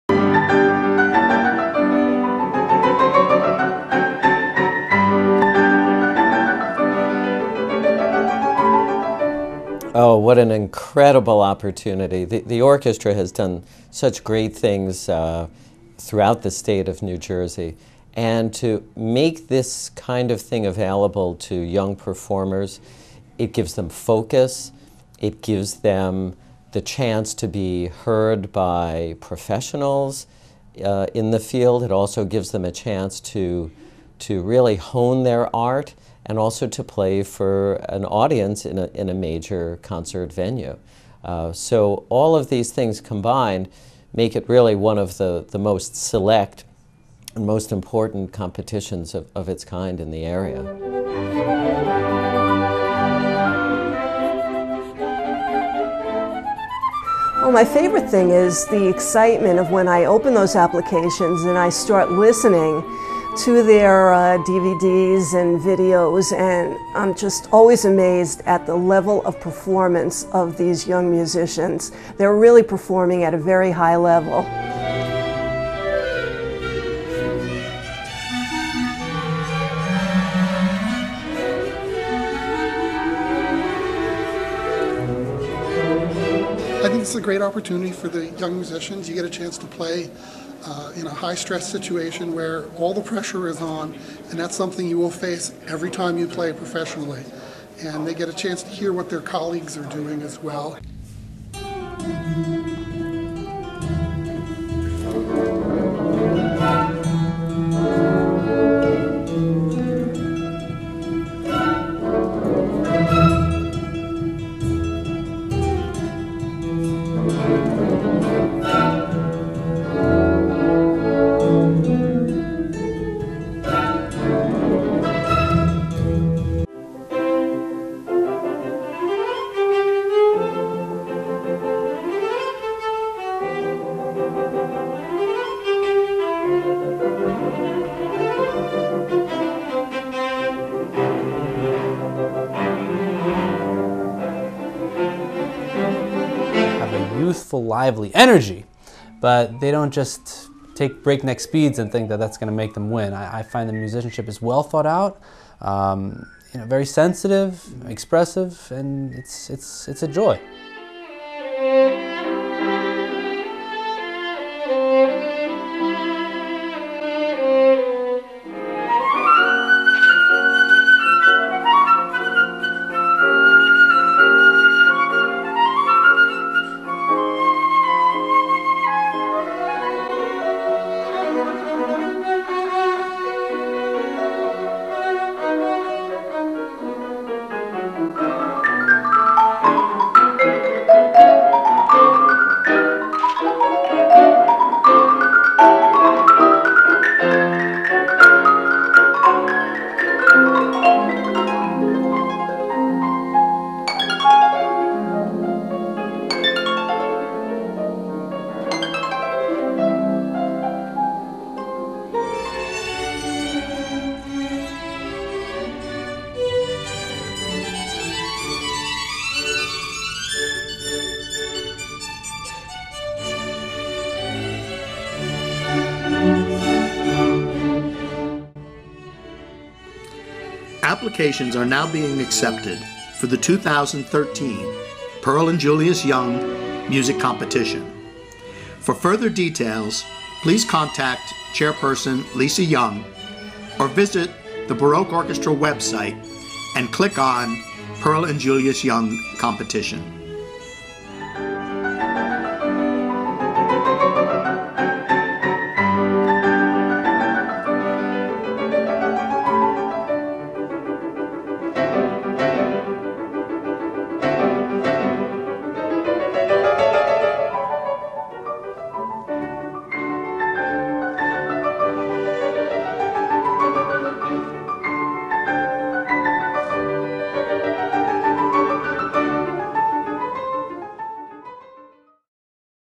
2013 Pearl & Julius Young Music Competition
2013 Pearl & Julius Young Music Competition, Lisa Young, Chairperson, Robert W. Butts, Artistic Director, The Baroque Orchestra of New Jersey,